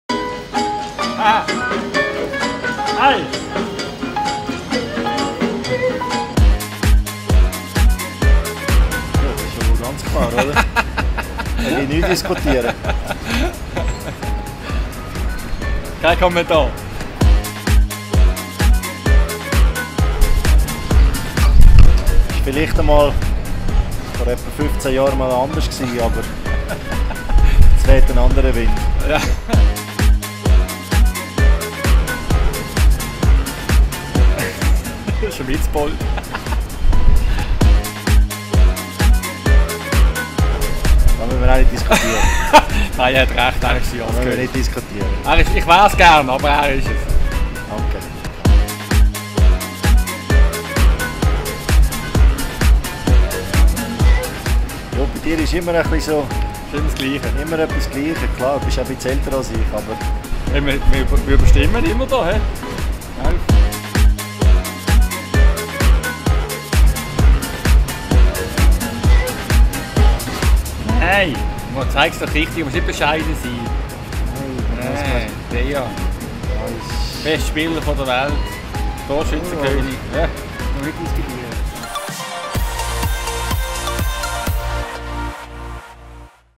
Poolmates: Dejan Stankovic vs Angelo Schirinzi
Roommates Edition Poolmates Paraguay 2019: Featuring Bomber Dejan Stankovic & General Angelo Schirinzi